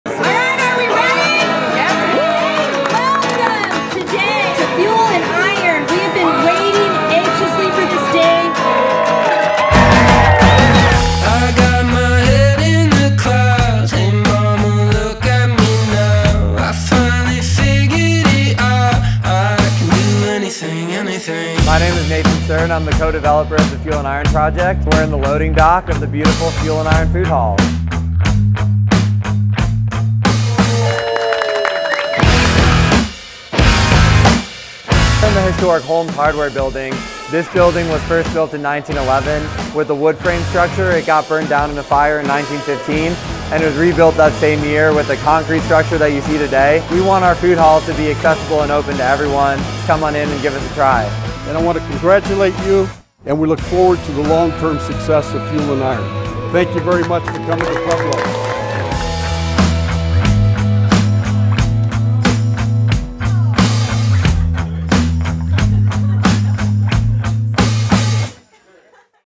FUEL & IRON GRAND OPENING: Pueblo Colorado Food Hall
FUEL & IRON GRAND OPENING: Pueblo Colorado's Food Hall located in the heart of Downtown Pueblo 🔥 Come visit Pueblo's newest attraction & enjoy a variety of delicious meals & drinks from: